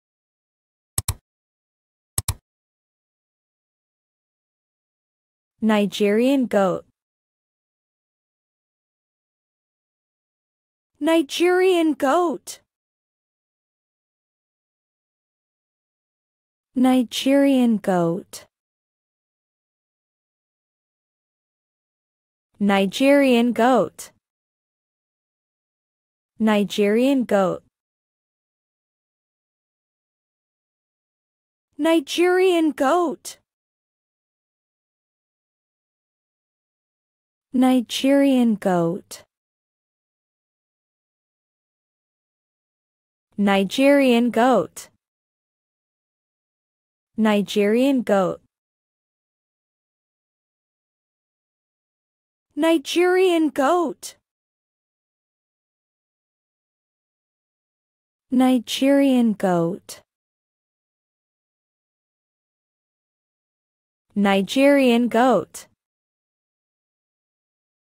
How to pronounce (Nigerian Goat) in English correctly
This video shows you how to say (Nigerian Goat). Listen and learn the English pronunciation of Nigerian Goat.

 The Nigerian dwarf is a miniature goat from West Africa. It has been domesticated as a dairy goat and can be found throughout the world. Goats are herbivores.